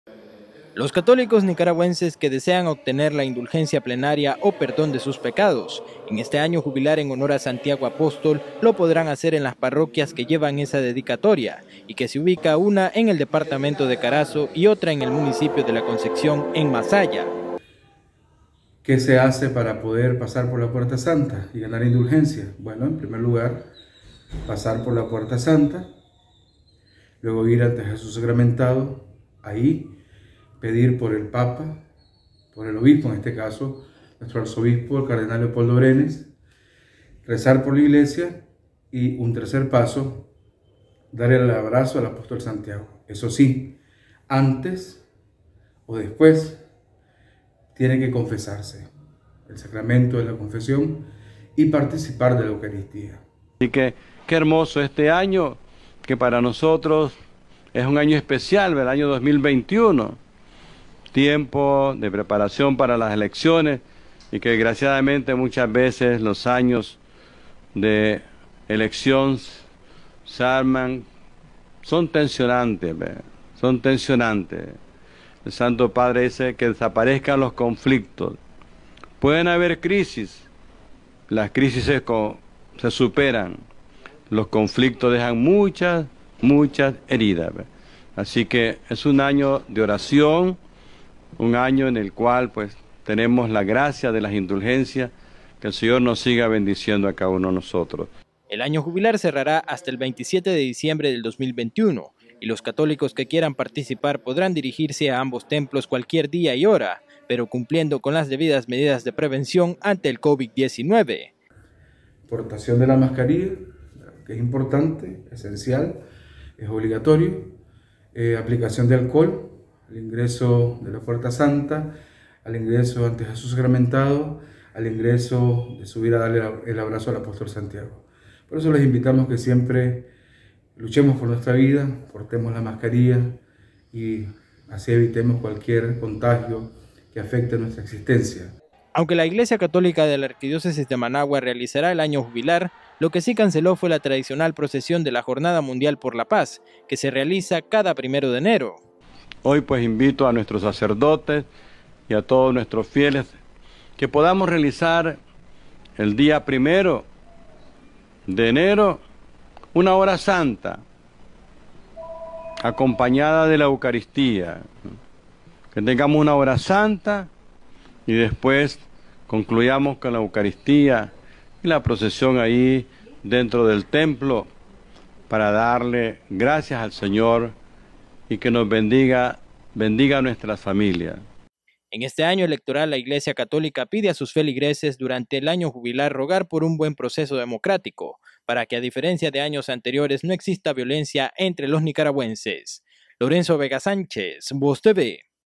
Iglesia Católica insta a los feligreses a obtener indulgencia plenaria este 2021
Este año jubilar en honor a Santiago Apóstol cerrará hasta el 27 de diciembre del 2021, cumpliendo con las medidas de prevención ante el Covid-19.